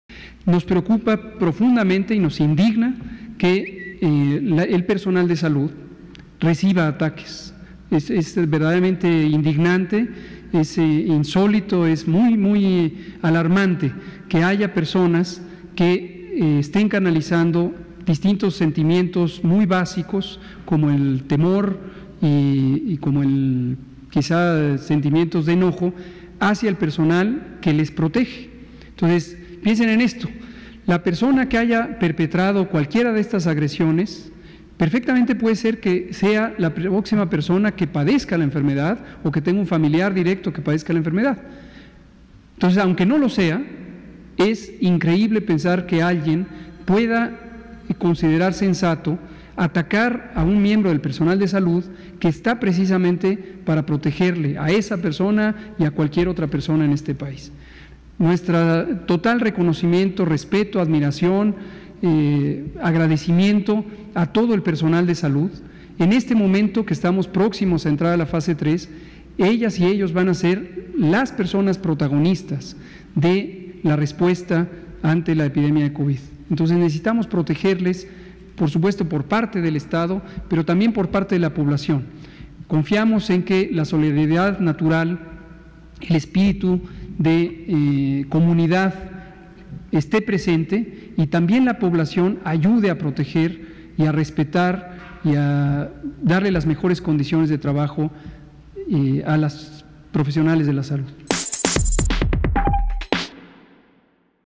Nos indignan los ataques al personal de Salud: López-Gatell
Hugo López-Gatell, subsecretario de Prevención y Promoción de la Salud, recalcó que es insólito y alarmante que existan personas que canalicen sus sentimientos hacia el personal de Salud, que les protege.